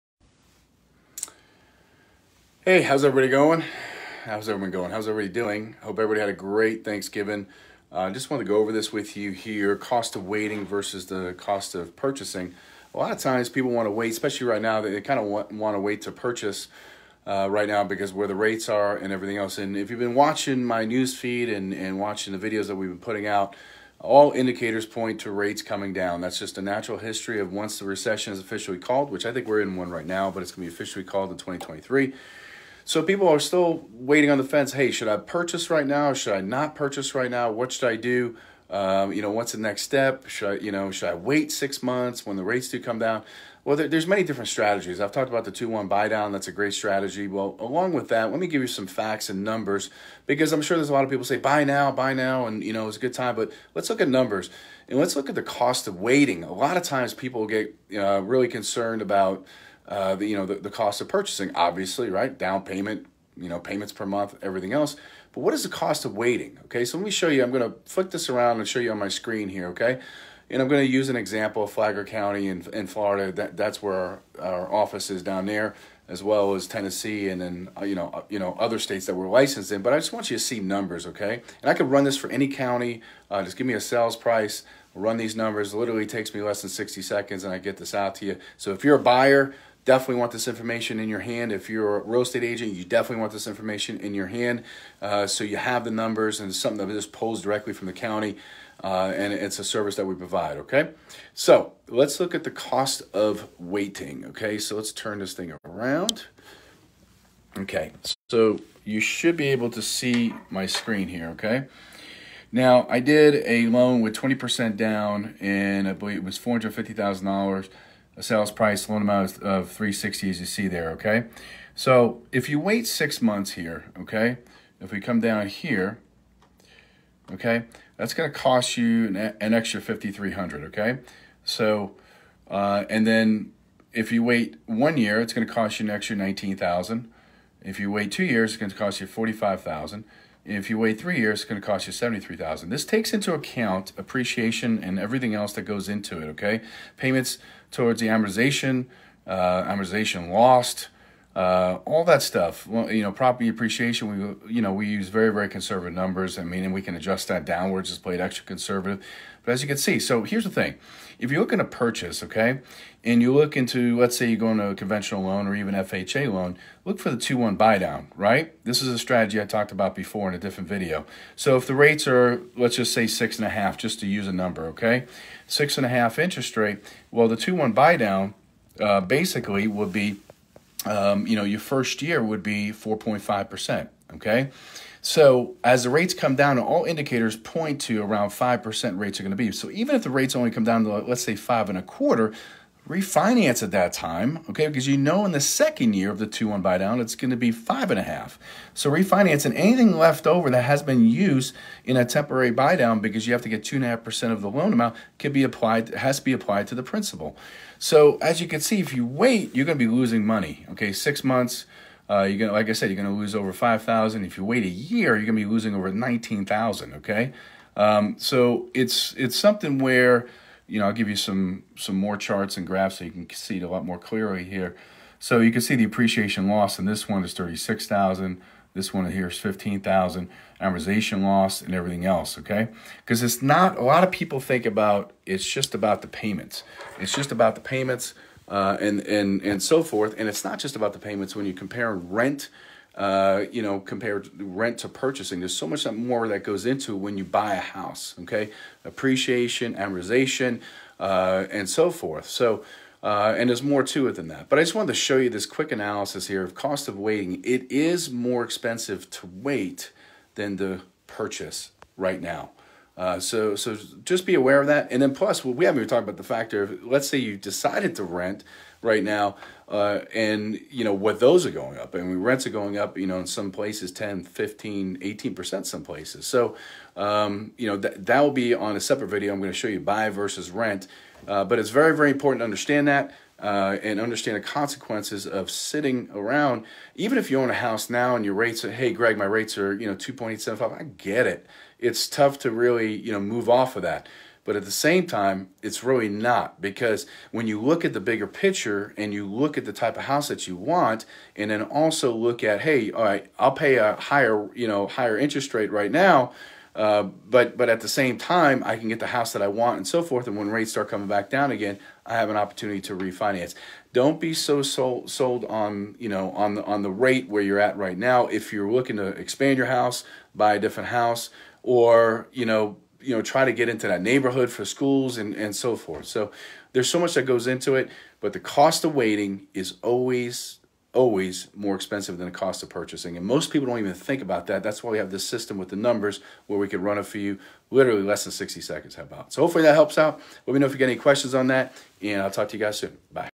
The Cost of Waiting is far more expensive than the Cost of Purchasing
when it comes to home buying let me explain